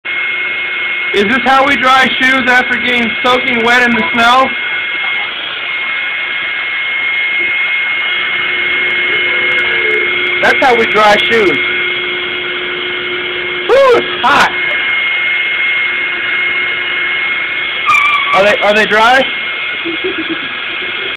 Shoes How to dry shoes in a hotel bathroom after soaking from snow
My friends daughter only brought one pair of shoes to play in snow.   after playing they were soaking wet.   we were hungry and needed to go to dinner. so i rigged up the bomb ass shoe dryer in the bathroom.   The bathroom stunk up a little..  lol